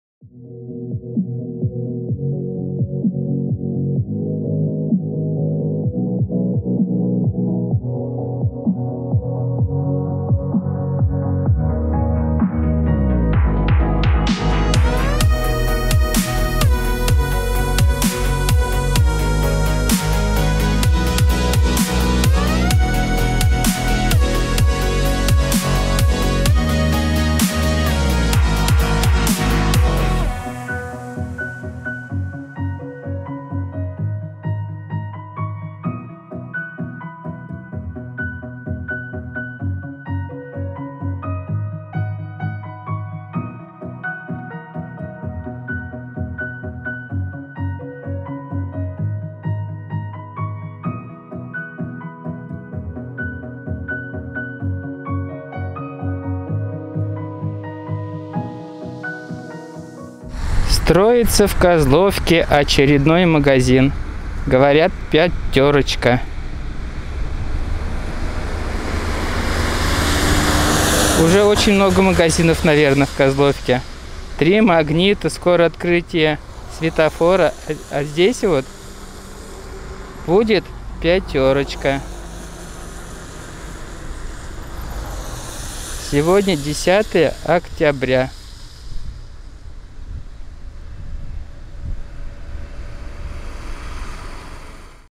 Ремонт дороги Строительство магазина Пятерочка Козловка 27 09 2019 и 10 10 2019
Ремонт дороги Строительство магазина Пятерочка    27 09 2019 и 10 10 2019 Ремонт дороги главная дорога центральная улица город  Козловка Строительство магазина Пятерочка идет быстрыми темпами хотят успеть к морозам